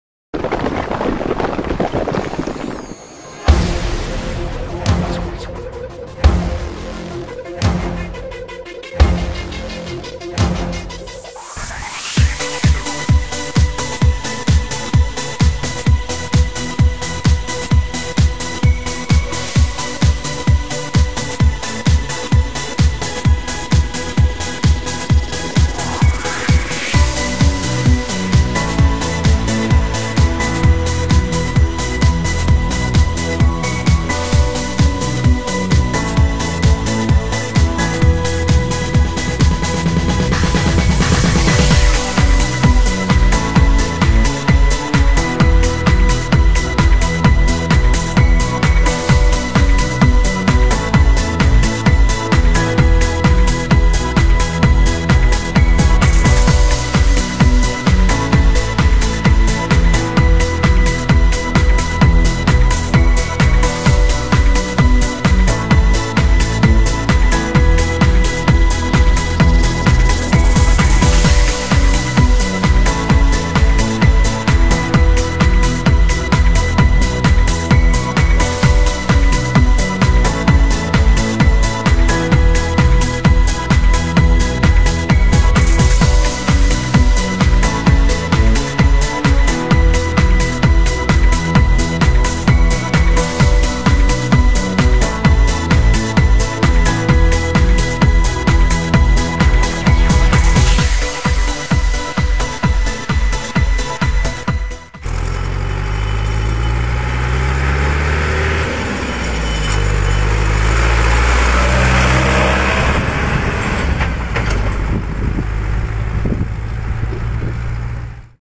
Nawozy 2014 #2 Ursus C 385 + Rauch MDS 935
Film taki sobie ponieważ nie miałem zbyt dużej ilości dobrego materiału więc jest krótko i na temat ! :) Ursus C 385 + Rauch MDS 936 oraz Zetor 8145 + tur.
Dawka nawozu to około 300 kg/ha, wielkości rozrzutu nie znam ponieważ nie pytałem ;)
Jeżeli jeszcze nie zaspokoiłem twoich pytań pisz !